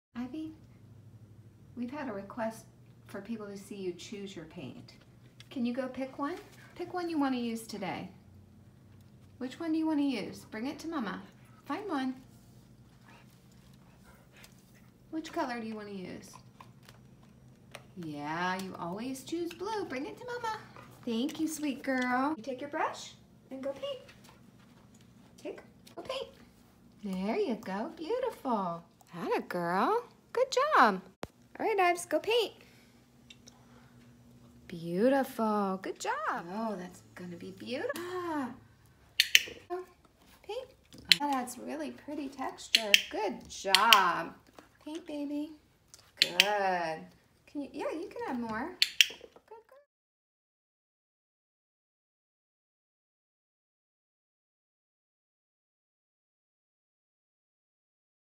Australian Shepherd - Canine Artist
Australian Shepherd painting abstract art to raise money for charity.